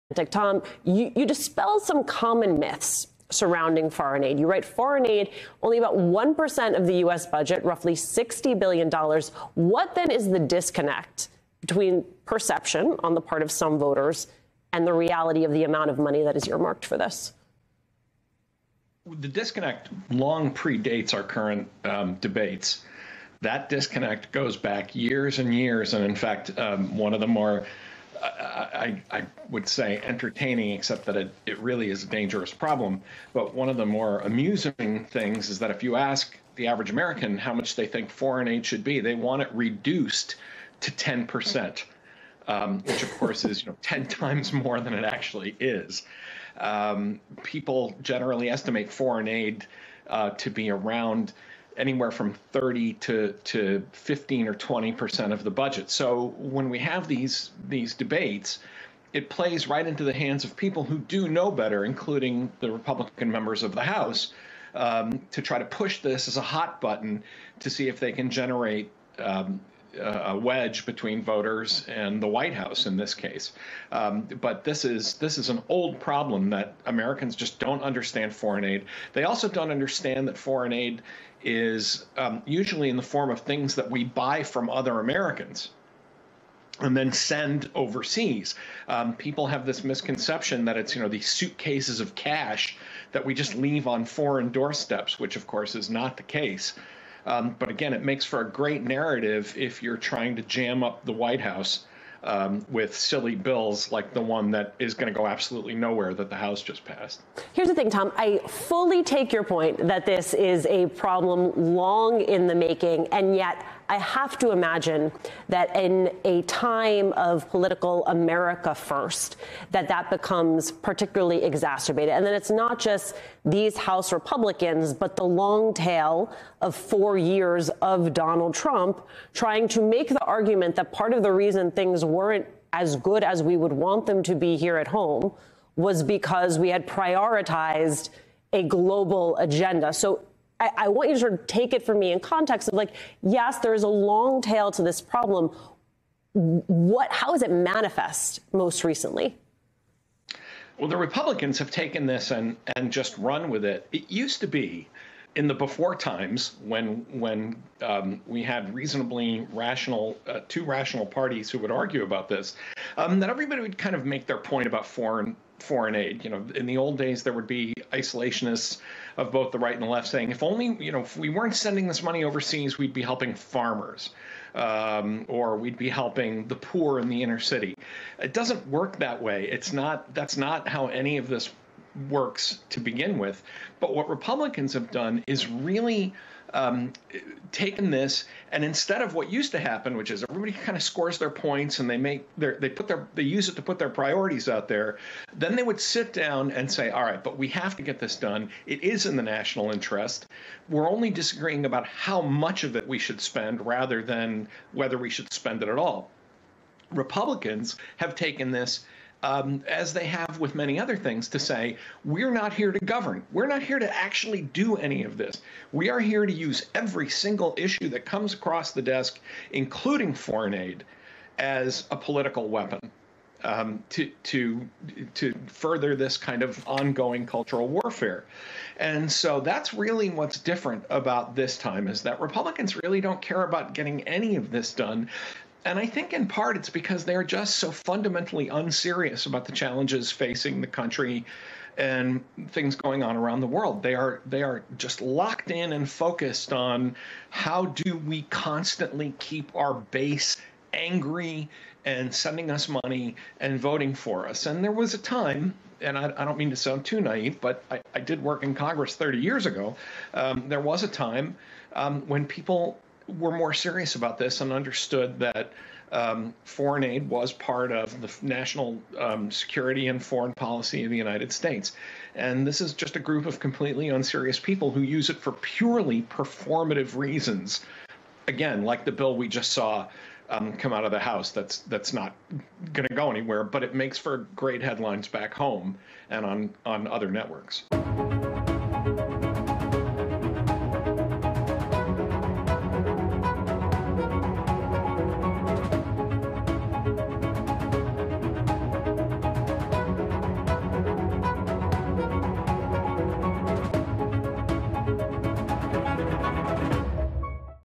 What modern Republican voters get wrong about the concept of foreign aid
It turns out that only 1% of the U.S. budget is spent on foreign aid. But far-right Republican lawmakers continue to push the narrative that the government spends too much on foreign aid, money that should be going to American interests. Tom Nichols from The Atlantic debunks that idea for American Voices host Alicia Menendez and explains how lawmakers should frame the conversation.